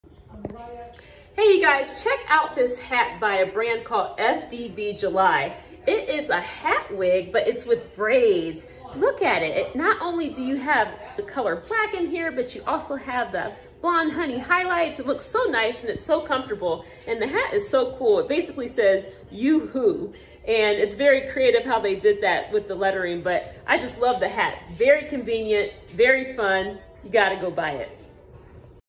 The secret to having flawless braids: FDBJulyy Baseball Cap braided wig
Do you want to know how to get that flawless braids look? Look no further. In this video, you will see me rock a cool, face-framing hairstyle that offers easy installation! This flawless braided hat wig will be perfect for you. The style is deceivingly simple to create. Watch how you can do it yourself!

Behind foreveryourgirl1814*: This channel represents all things fun in hair and fashion!